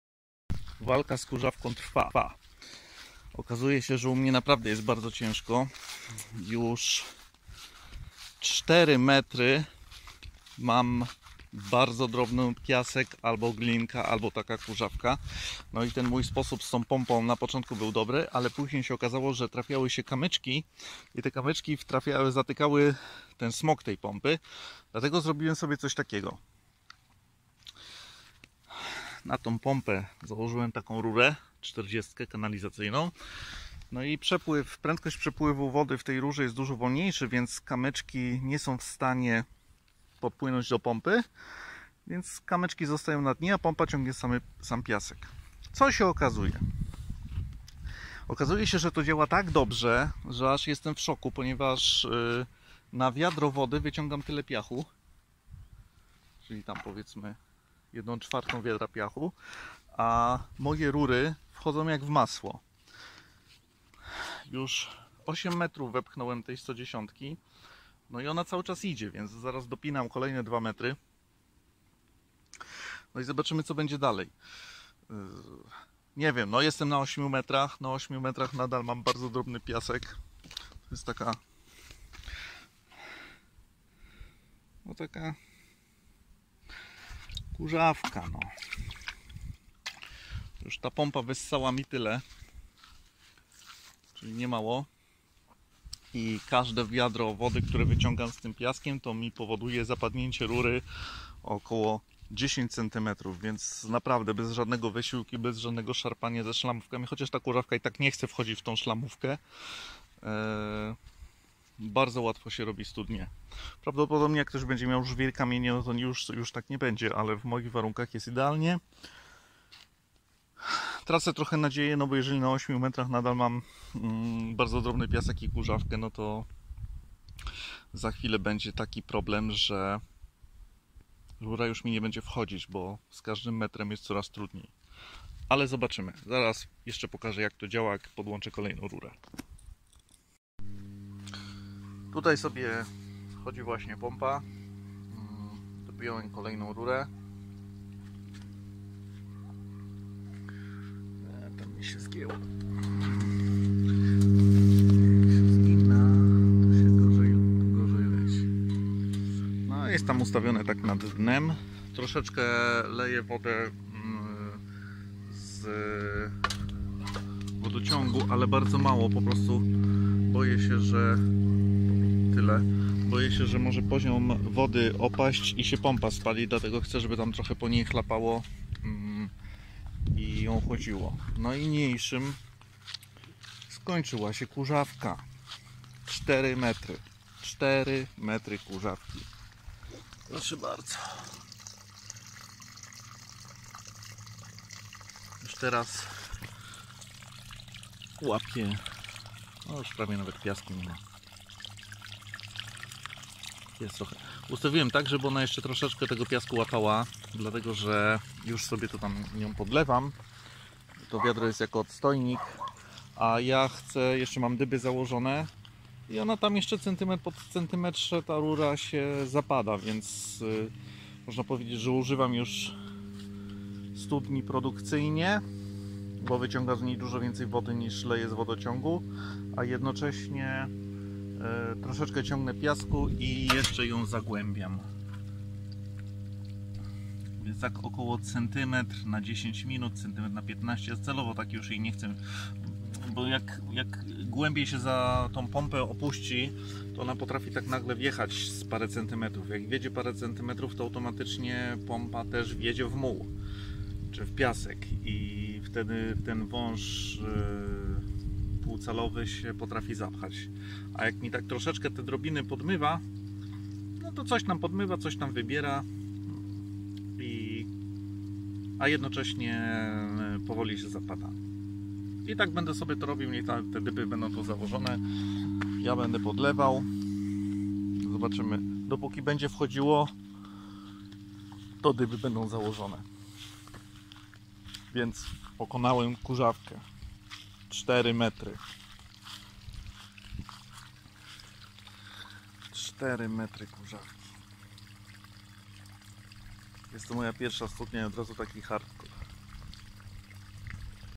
Studnia DIY - wygrałem z hardkorową kurzawką.
Wygrałem z kurzawką!! Nie znam się ale wg mnie 4 metry kurzawki to nie jest normalne zjawisko :)